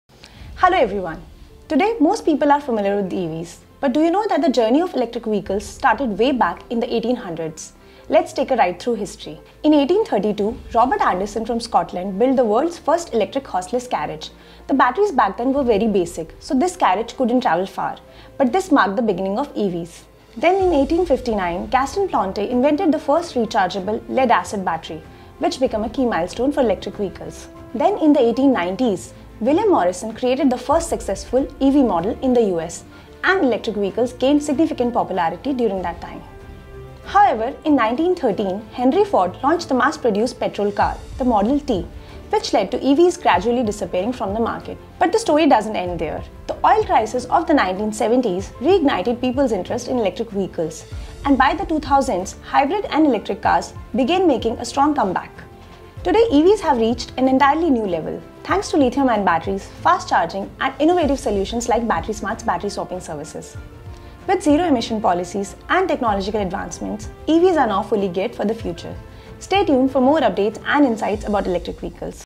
Did you know electric vehicles date back to the 1800s?
Watch this video to discover the incredible journey of electric vehicles from early inventions to today’s high-tech EVs driving the future! 🌍